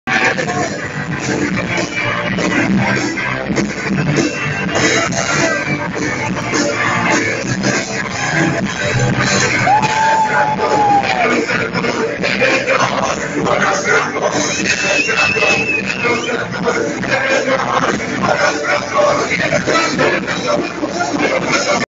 KLAUN STAGE DIVING - PUNKOVE KRALOVSTVI 04/2015
Klaun ala vožralej strejc se neohroženě po zádech vrhá do rozvášněného davu punkýšů a jiných individuí! SLÁVA HURÁ!